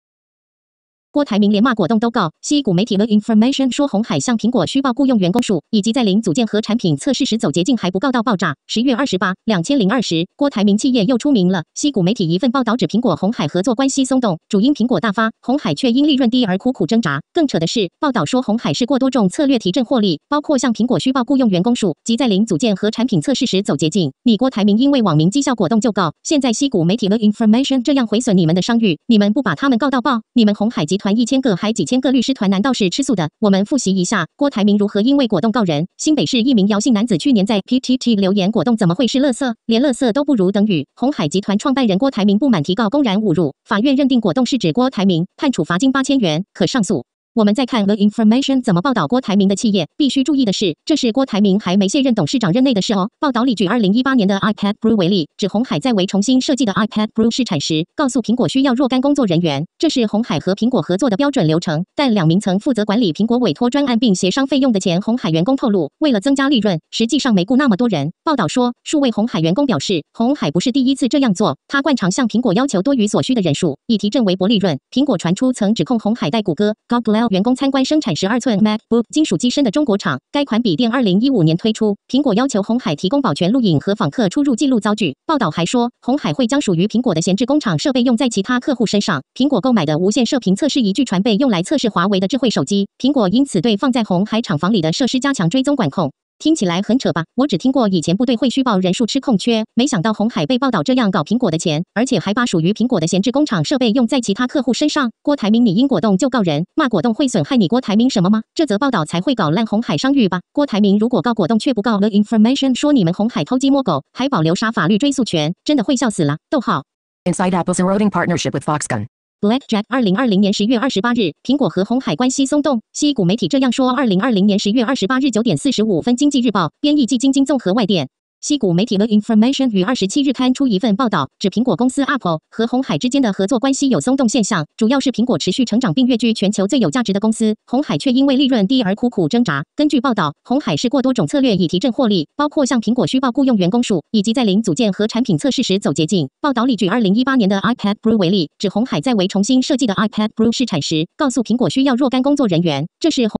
郭台銘連罵果凍都告，矽谷媒體The Information說鴻海向蘋果虛報雇用員工數，以及在零組件和產品測試時走捷徑還不告到爆炸！？
10月 28, 2020
​郭台銘企業又出名了，矽谷媒體一份報導指蘋果鴻海合作關係鬆動，主因蘋果大發，鴻海卻因利潤低而苦苦掙扎。更扯的是，報導說鴻海試過多種策略提振獲利，包括向蘋果虛報雇用員工數，及在零組件和產品測試時走捷徑。你郭台銘因為網民譏笑果凍就告，現在矽谷媒體The Information這樣毀損你們的商譽，你們不把他們告到爆？你們鴻海集團一千個還幾千個律師團難道是吃素的？？

我們複習一下，郭台銘如何因為果凍告人：
新北市一名姚姓男子去年在PTT留言「果凍怎麼會是垃圾？連垃圾都不如」等語，鴻海集團創辦人郭台銘不滿提告公然侮辱。法院認定「果凍」是指郭台銘，判處罰金8000元，可上訴。

我們再看The Information怎麼報導郭台銘的企業，必須注意的是，這是郭台銘還沒卸任董事長任內的事哦：

報導裡舉2018年的iPad Pro為例，指鴻海在為重新設計的iPad Pro試產時，告訴蘋果需要若干工作人員。這是鴻海和蘋果合作的「標準流程」，但兩名曾負責管理蘋果委託專案並協商費用的前鴻海員工透露，為了增加利潤，實際上沒雇那麼多人。
報導說，「數位鴻海員工表示，鴻海不是第一次這樣做。它慣常向蘋果要求多於所需的人數⋯⋯以提振微薄利潤。」
蘋果傳出曾指控鴻海帶谷歌（Google）員工參觀生產12吋MacBook金屬機身的中國廠。該款筆電2015年推出。蘋果要求鴻海提供保全錄影和訪客出入記錄遭拒。
報導還說，鴻海會將屬於蘋果的「閒置工廠設備」用在其他客戶身上。蘋果購買的無線射頻測試儀據傳被用來測試華為的智慧手機。蘋果因此對放在鴻海廠房裡的設施加強追蹤管控。

聽起來很扯吧，我只聽過以前部隊會虛報人數吃空缺，沒想到鴻海被報導這樣搞蘋果的錢，而且還把屬於蘋果的「閒置工廠設備」用在其他客戶身上？郭台銘你因果凍就告人，罵果凍會損害你郭台銘什麼嗎？這則報導才會搞爛鴻海商譽吧！

郭台銘如果告果凍卻不告The Information說你們鴻海偷雞摸狗，還保留啥法律追訴權，真的會笑死啦！

Inside Apple’s Eroding Partnership with Foxconn

Blackjack 2020/10/28

蘋果和鴻海關係鬆動？矽谷媒體這樣說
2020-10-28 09:45 經濟日報 / 編譯季晶晶／綜合外電